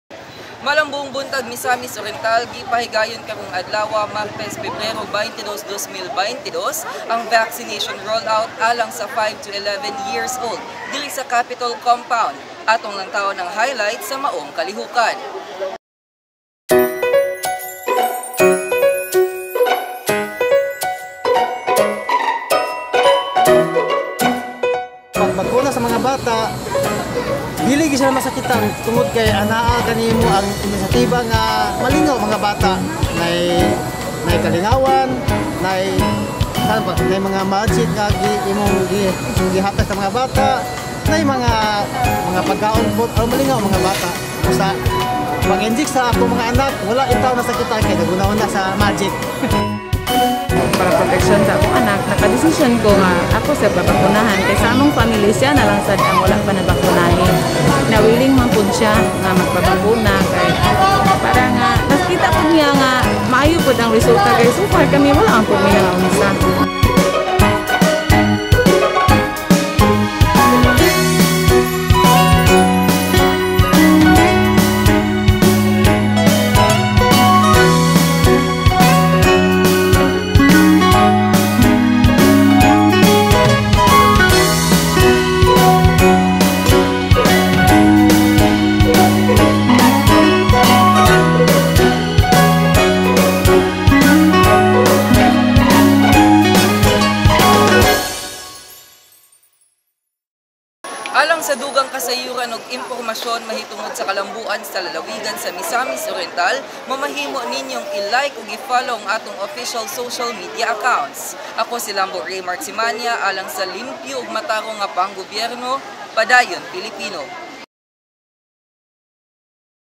Unang hugna sa vaccination roll-out alang sa mga kabataan nag-edad 5-11 anyos nagmalampuson
Nagmalampuson ang panggamhanan sa Misamis Oriental sa pagpanguna sa Provincial IATF human napahigayon ang unang hugna sa vaccination roll-out gamit ang reformulated Pfizer alang sa mga kabataan nag-edad 5-11 anyos.